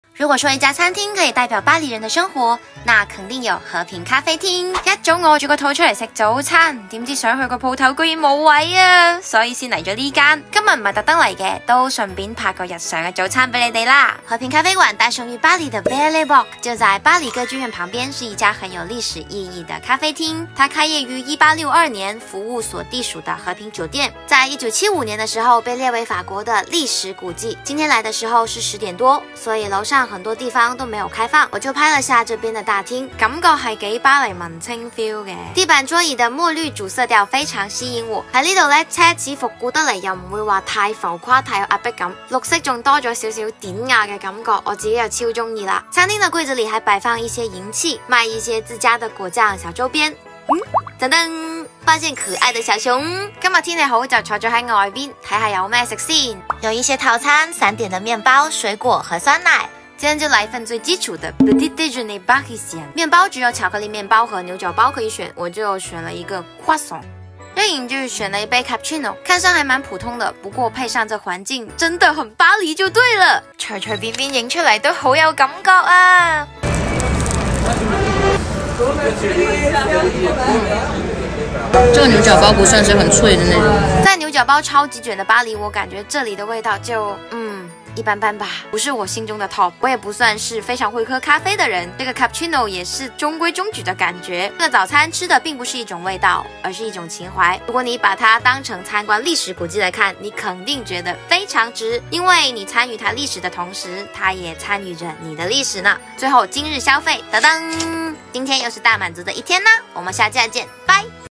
在“100年前”的巴黎咖啡廳吃早餐是什麼體驗？【Uta食間#06】
★訂閱Utatv（每週更新）:
謝謝你們的訂閱和支持！

咖啡廳是巴黎歌劇院旁邊的「和平咖啡廳」
感覺有時間的話，慢慢喝杯咖啡吃個牛角包🥐，欣賞街邊的景緻還是很不錯的。不過我還沒正式到裡面用過正餐，用過餐的小姐妹可以多多留言一起分享一下體驗吶！最後附上地址：

「Café de la Paix」
5 Pl. de l'Opéra, 75009 Paris

▶小紅書：Uta醬巴黎live

【Uta的關鍵字】
法國 / 法國文化 / 法國留學 / 巴黎 / 開箱 / 旅行 / 探店 / 美食 /  購物 / vlog